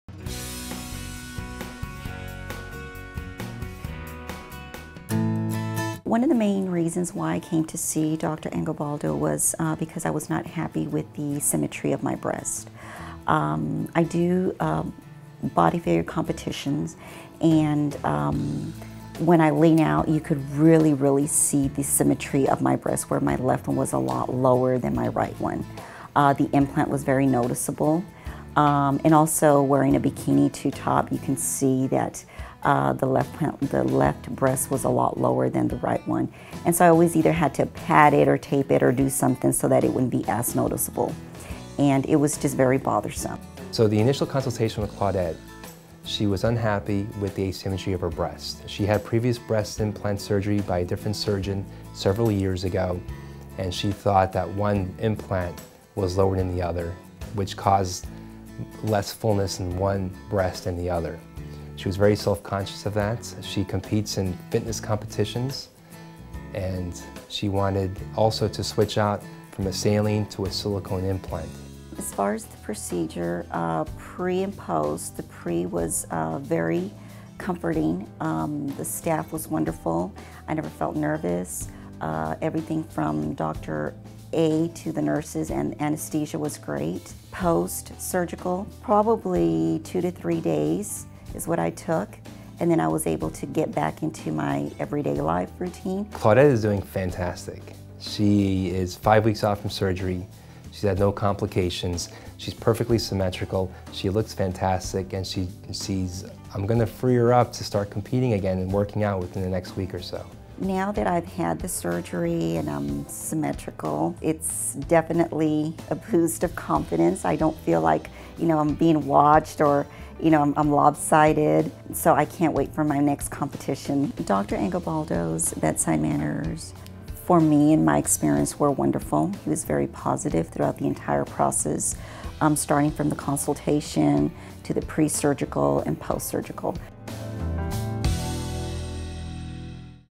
Breast Implant Exchange with Plastic Surgeon Dr. Jeff Angobaldo
Dallas plastic surgeon Dr. Jeff Angobaldo with Renaissance Plastic Surgery, talks about the breast implant exchange procedure. Claudette was not happy with the symmetry of her breasts and needed Dr. Angobaldo to correct this problem.